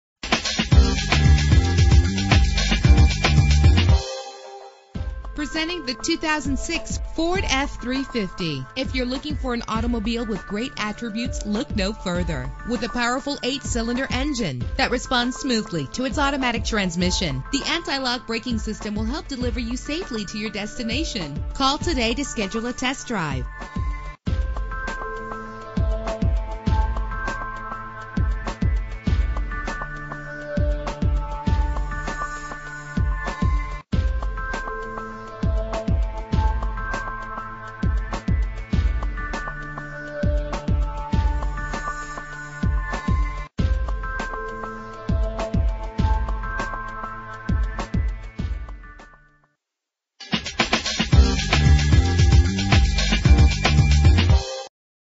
2006 Ford F-350 - Murray UT
Year: 2006
Make: Ford
Model: F-350
Trim: Super Duty
Engine: 6.0 liter 8 cylinder 32 valve
Transmission: Automatic
Color: Brown
Mileage: 92457
Address:
4315 S State Street